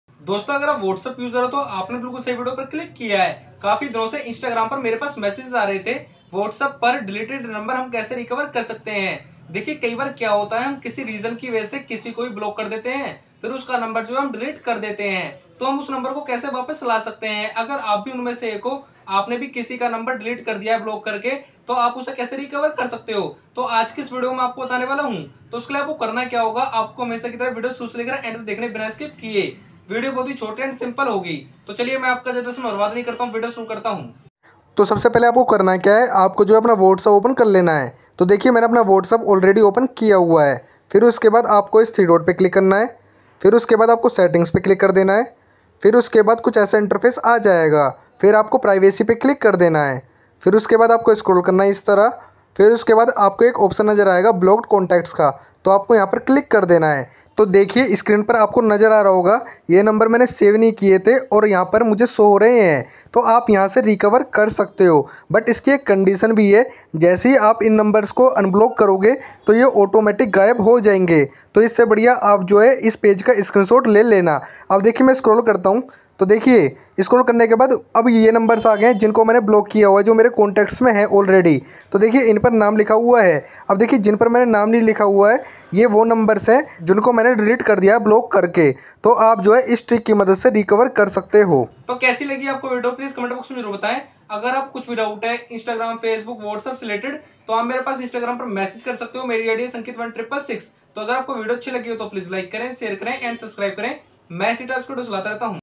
Whatsapp ke delete number wapas kaise laye | how to recover deleted number in whatsapp
Dosto aj ki is video m mane btaya hai Whatsapp ke delete number wapas kaise laye | how to recover deleted number in whatsapp